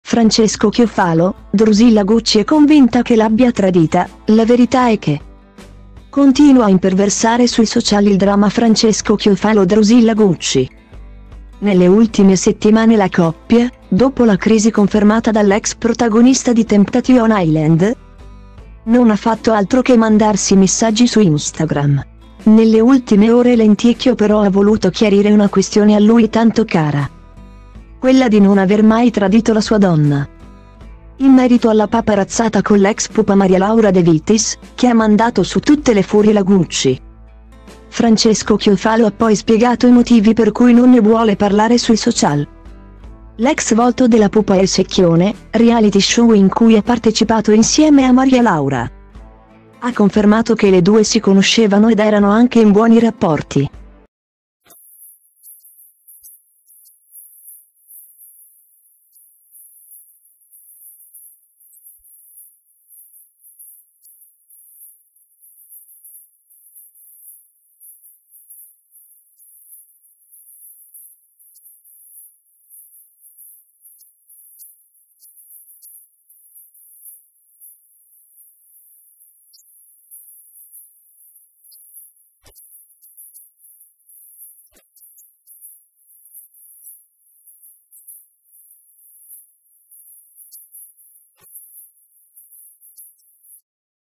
Francesco Chiofalo: “Drusilla Gucci è convinta che l’abbia tradita, la verità è che…”
Continua a imperversare sui social il drama Francesco Chiofalo – Drusilla Gucci. Nelle ultime settimane la coppia, dopo la crisi confermata dall’ex protagonista di Temptation Island,non ha fatto altro che mandarsi messaggi su Instagram. Nelle ultime ore Lenticchio però ha voluto chiarire una questione a lui tanto cara,